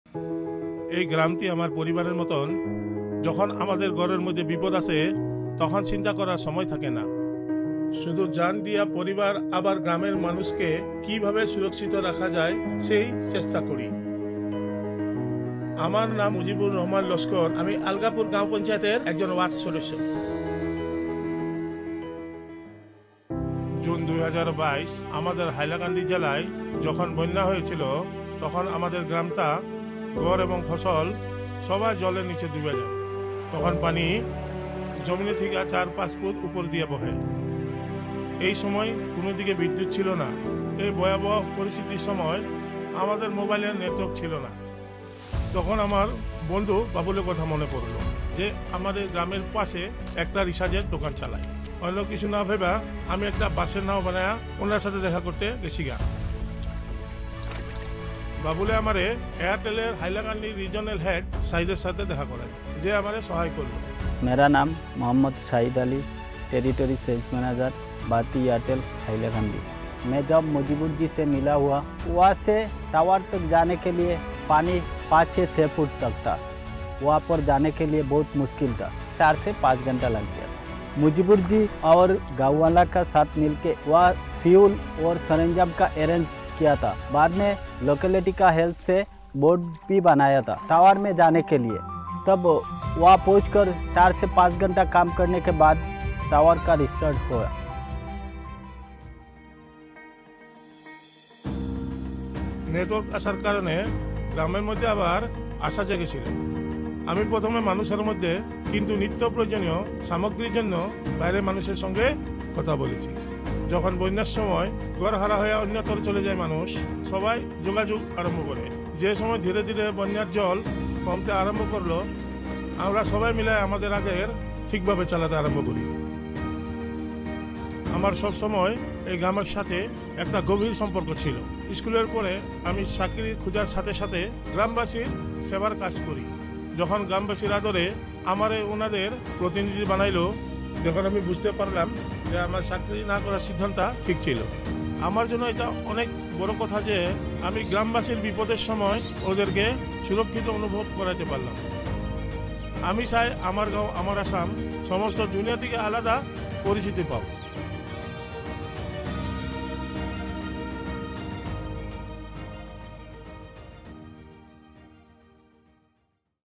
Meet Mojibur | Who Connected His Village To The World | English | Latest Update
Meet Mojibur Rehman Lashkar a ward member of the Algapur Gram Panchayat, whose dedication and never give up attitude helped connect his village to the world during a time of crisis.

Watch the video to know his full story.